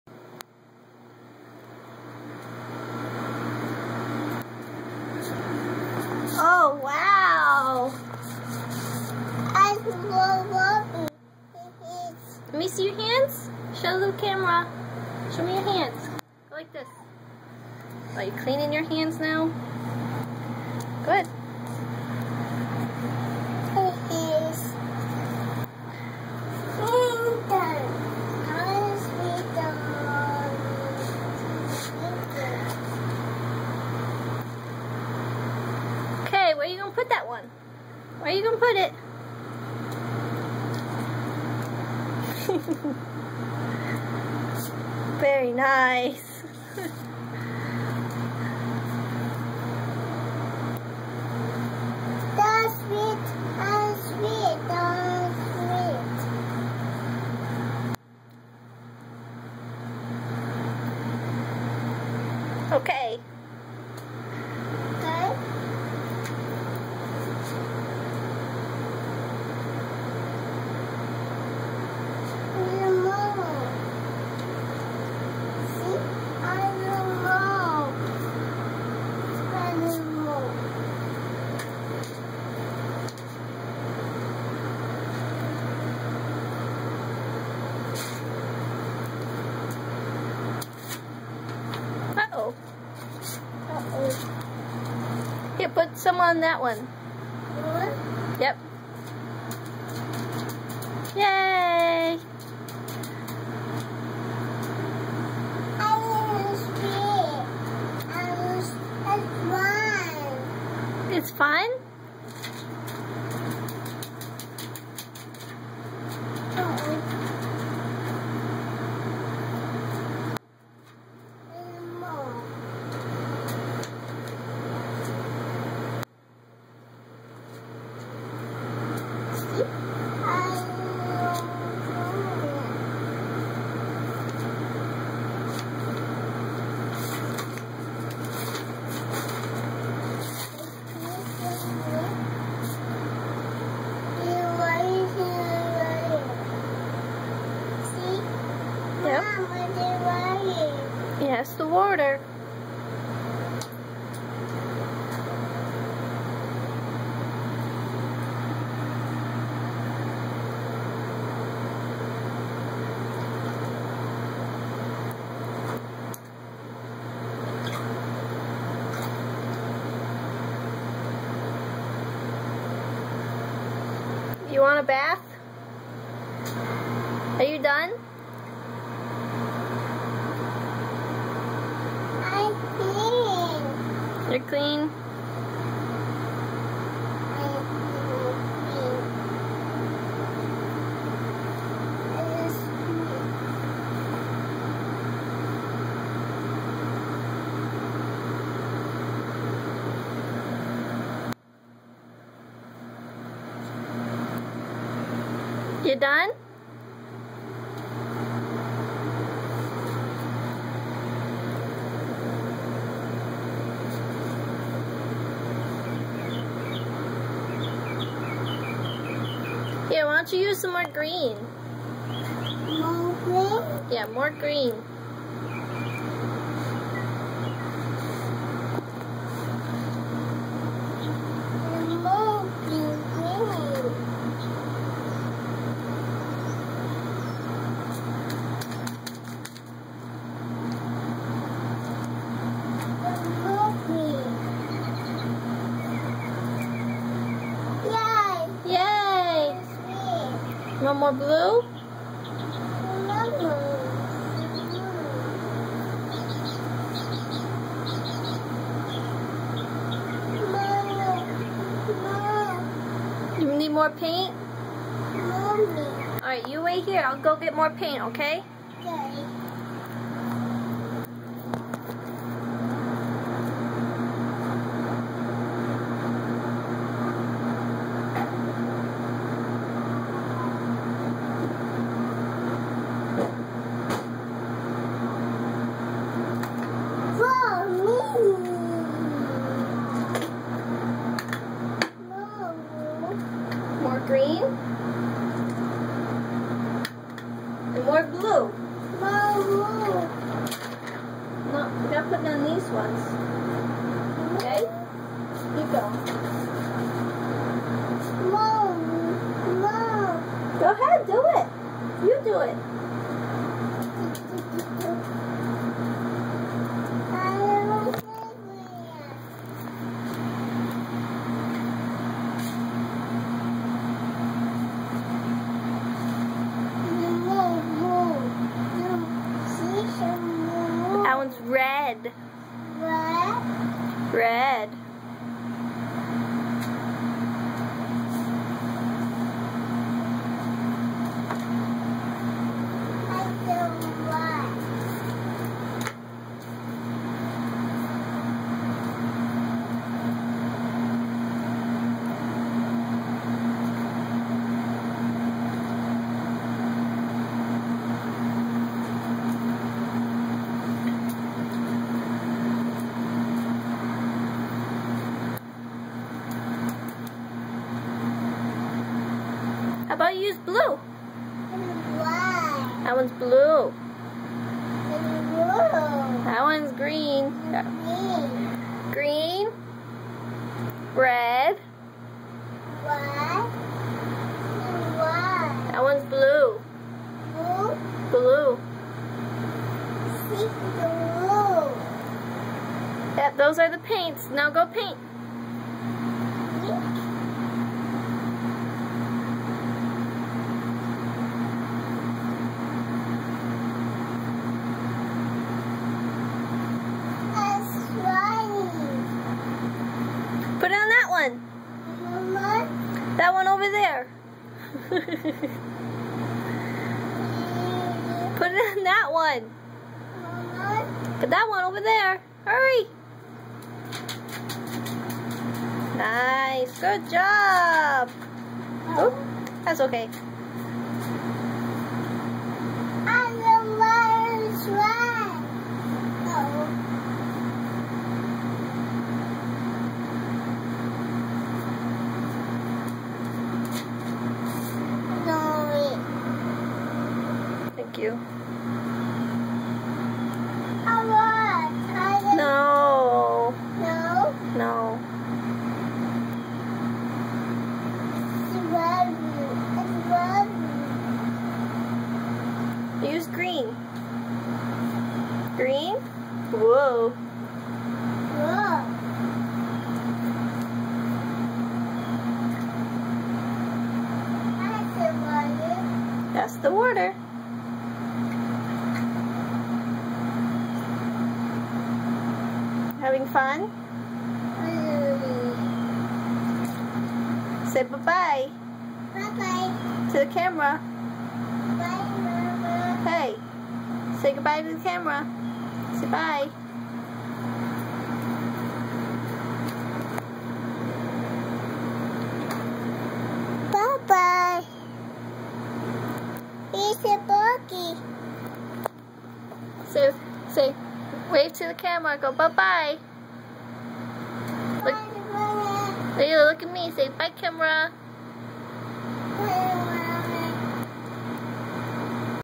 Finger & Toe Painting with Lilu!
My daughter decided using just fingers was not good enough.